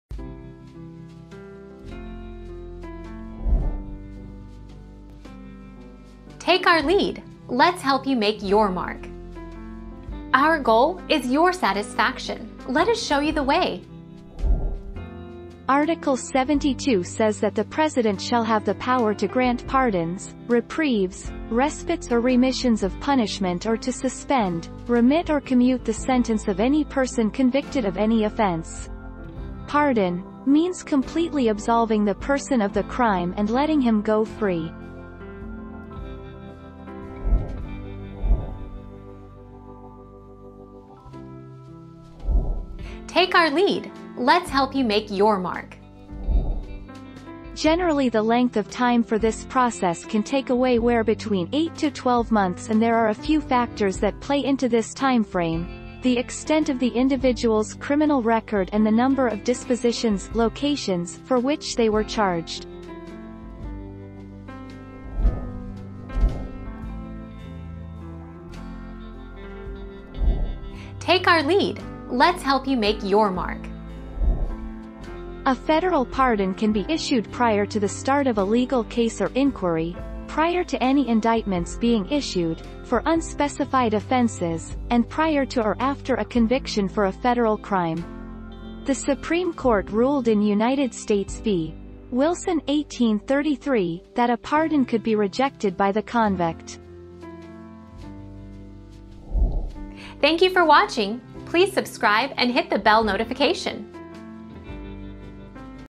What happens when a person is pardoned?
00:00 - What happens when a person is pardoned?
00:39 - How long does it take to get a pardon?
01:06 - Can a president be pardoned before being charged?

Laura S. Harris (2021, January 14.) What happens when a person is pardoned?
    AskAbout.video/articles/What-happens-when-a-person-is-pardoned-210606